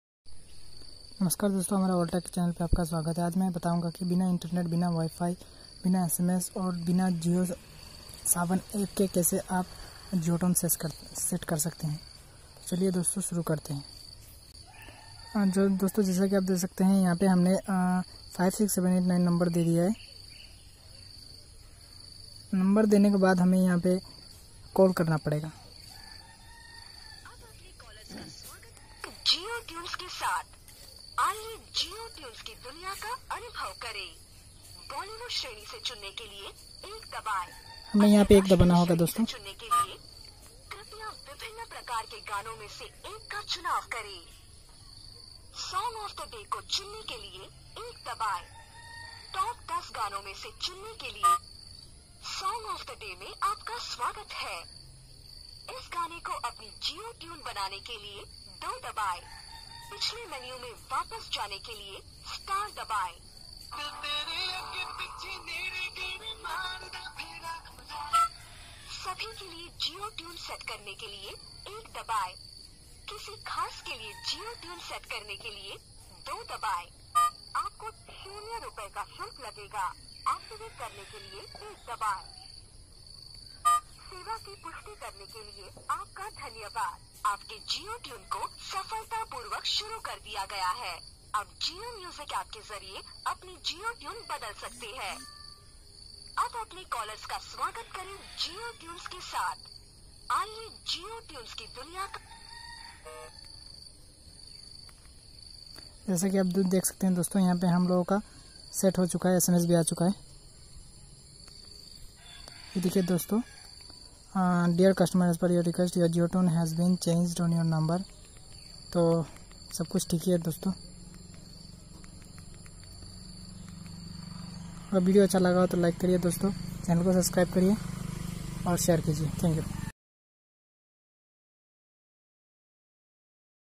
How to set jio tune without internet or SMS# || Using IVR call
You Can set Jio tune Using IVR call
Using  56789 this no.